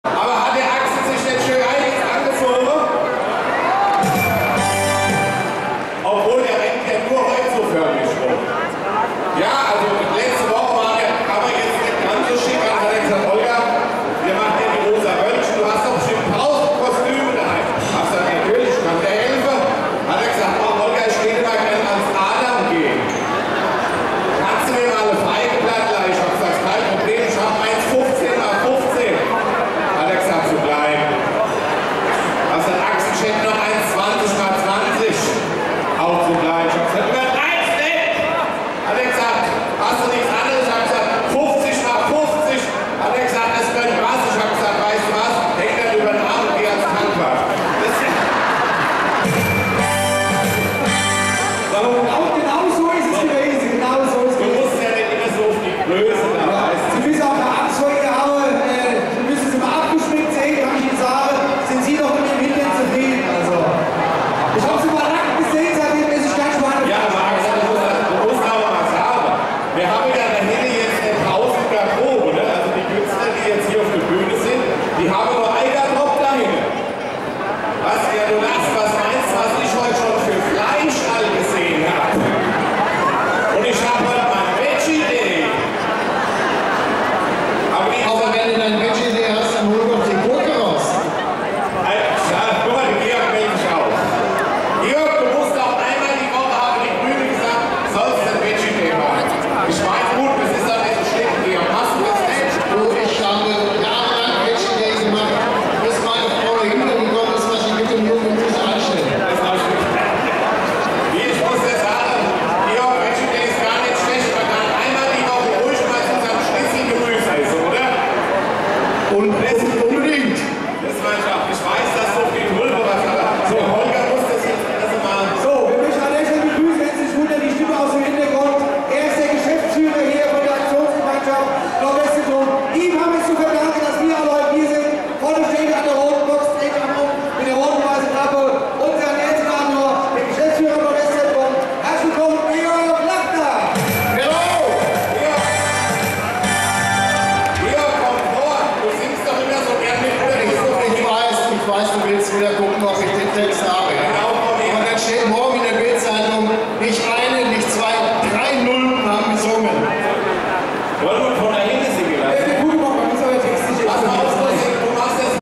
Nordwestzentrum Frankfurt Geschwister Hofmann Olga Orange Prinzenpaar 5
Nordwestzentrum Frankfurt am Main Auftritt im Rahmen der Faschingszeit Vor Ort ab Veranstaltungsbeginn: 15.11 Uhr Veranstaltungsort: Große Bühne im NWZ Auftritt A & A ca. 16 Uhr Weitere Mitwirkende: Prinzengarde und weitere regionale Künstler Eintritt: frei! Also nun viel Spaß am 28.02. in Frankfurt Hubert Fella und Matthias Mangiapane Onlinebuchung @

Nordwestzentrum Frankfurt Geschwister Hofmann Olga Orange Prinzenpaar am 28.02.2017 Hubert Fella und Matthias Mangiapane